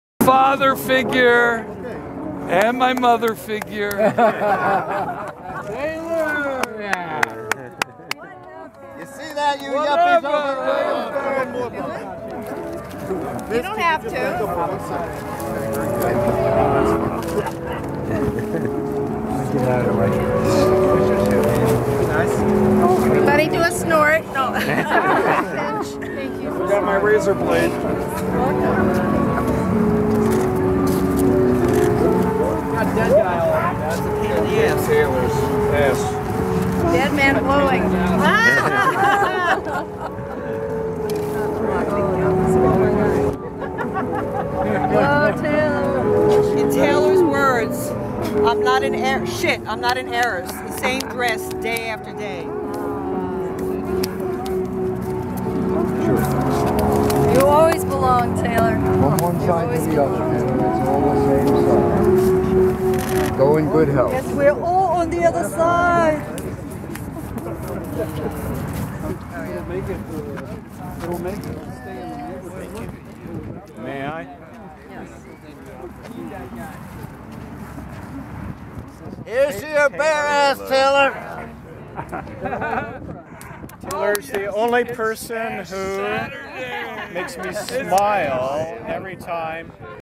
TAYLOR MEAD BY THE FISTFUL
Friends took turns, each throwing a fistful of Taylor Mead's ashes into the East River.  It was a charming ceremony carried out with music chosen by Taylor himself.